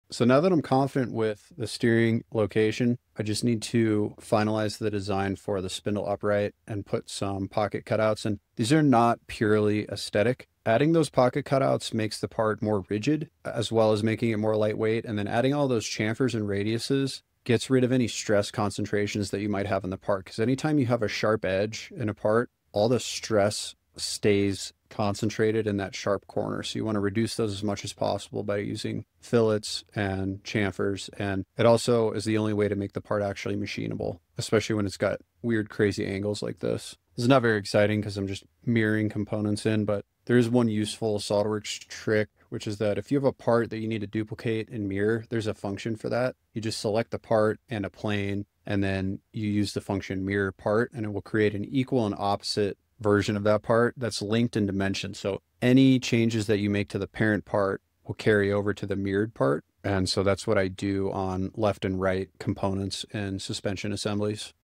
Designing the Spindle Upright: Enhancing Rigidity and Reducing Weight
Now that the steering location is confirmed, I'm finalizing the spindle upright design. I'm adding pocket cutouts for both lightweighting and increased rigidity—they help form an I-beam structure. Crucially, I'm adding chamfers and radii everywhere to eliminate stress concentrations; sharp corners concentrate stress, so rounding them out improves the part's durability and makes it machinable, especially with these complex angles.

A helpful SolidWorks trick for left/right components: use the Mirror Part function. You select the original part and a plane, and it creates a mirror image that is dimensionally linked. Any change to the parent part automatically carries over to the mirrored part, which is a huge time saver for suspension assemblies!